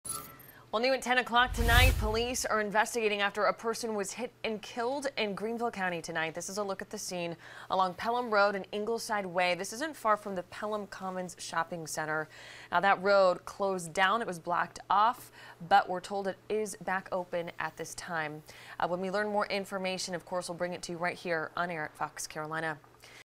Pedestrian hit and killed along Pelham Road in Greenville County
Authorities are investigating after a pedestrian was fatally hit along Pelham Rd. and Ingleside Way Thursday.  The road was temporarily closed as a result